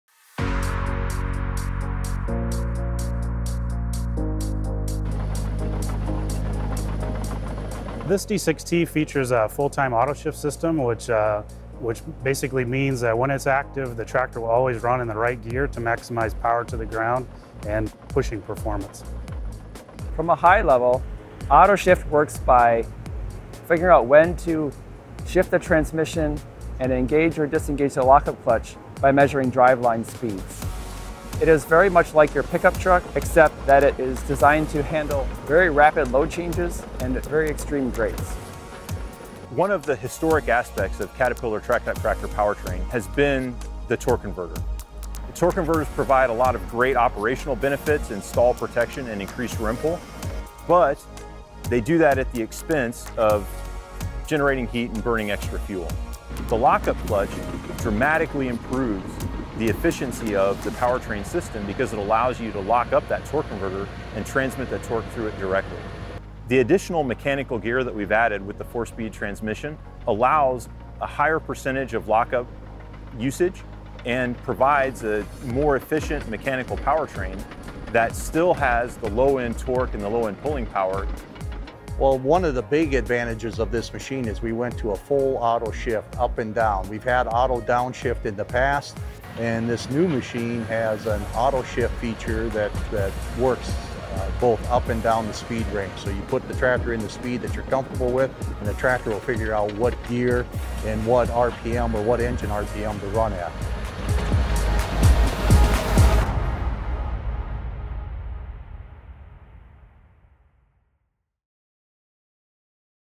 Engineering the Cat® D6T Bulldozer– Full Time Auto Shift
Caterpillar engineers talk about how they built on some proven concepts to develop a D6T bulldozer powertrain that’s durable, smooth and significantly more fuel efficient.